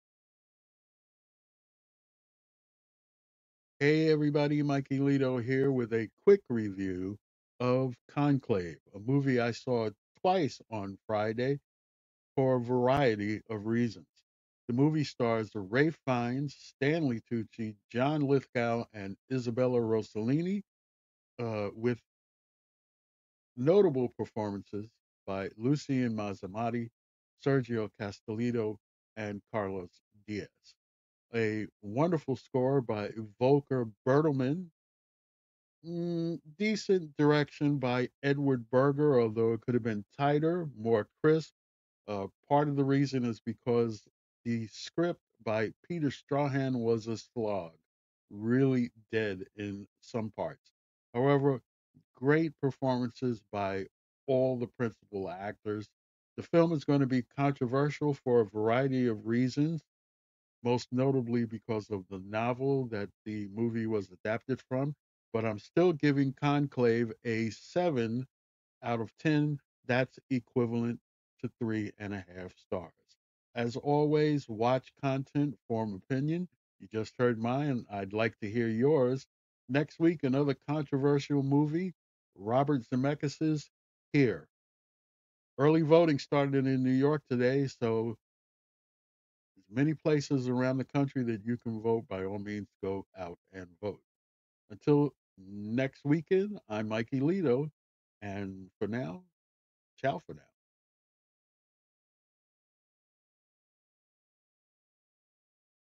CONCLAVE | Mikeylito's Matinée (Short)
The Pope is dead and director Edward Berger takes you inside how the new Pontiff is selected, based on the novel by Robert Harris. Mikeylito gives you his opinion about THE MOVIE, and not about any controversy surrounding it.

For up-to-date information about the channel,
consult the Community tab here on YouTube.
Also, follow Mikeylito on the social media service,  X.